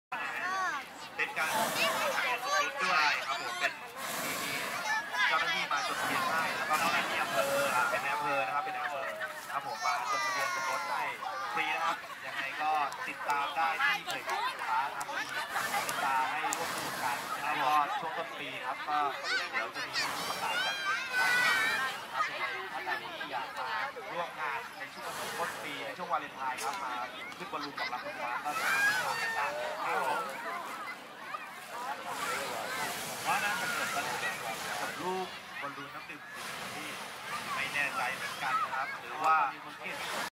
Free Stock Footage - Balloon and Cloud at Farm Festival 2019, 60p (Editorial)
Chiang Rai, Thailand - December 1, 2019: The traveler looking at the Balloon in Farm Festival 2019 at Singha Park Chiang Rai.